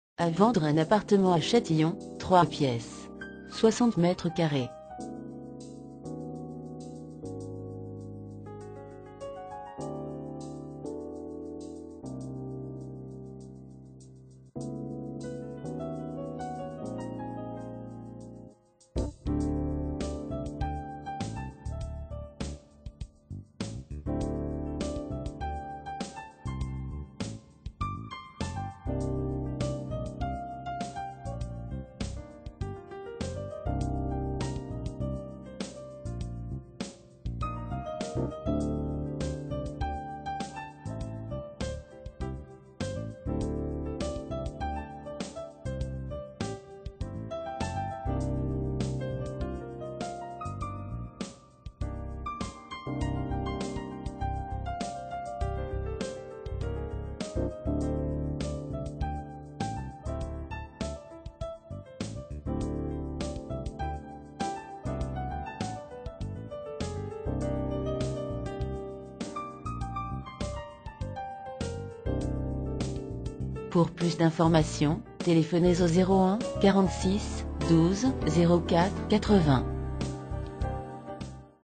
Achat Vente Appartement CHATILLON 92320 - 60 m2
Cliquez sur ce lien pour en savoir plus (prix, contacts)

A vendre à CHATILLON un appartement de 60 m2.

L agence Le TUC CHATILLON vous propose de nombreux autres biens immobiliers. Téléphonez au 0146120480 pour en savoir plus.

Annonce immobilière : Achat Vente Appartement CHATILLON 92320 Hauts de Seine